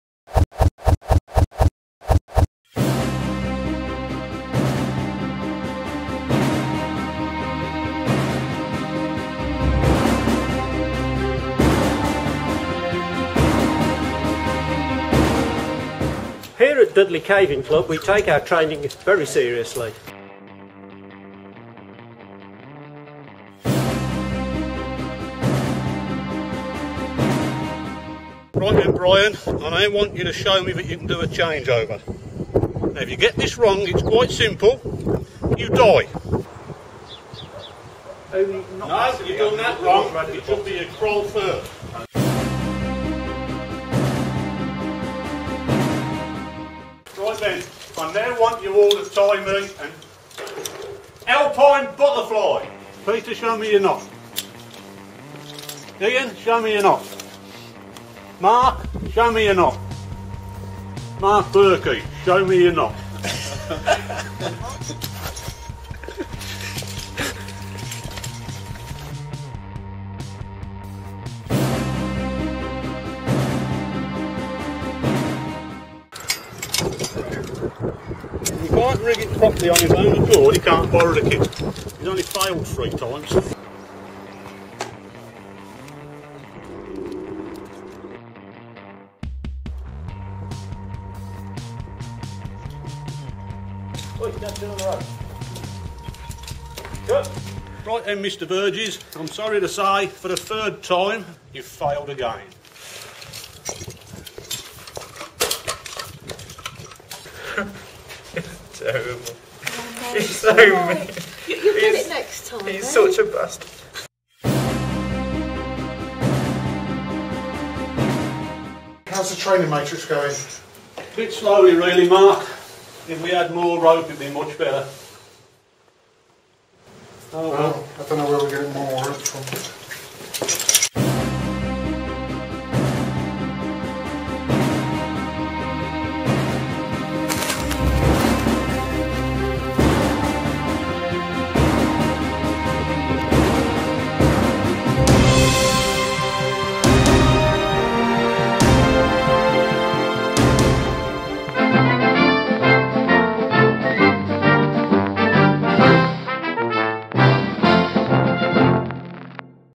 The Matrix - Dudley Caving Club Take Training Seriously!
A typical training evening at Dudley Caving Club's HQ. This is Dudley Caving Club's entry for UK Caving's competition to win 300 metres of SpanSet Gold rope. Full details of the competition can be found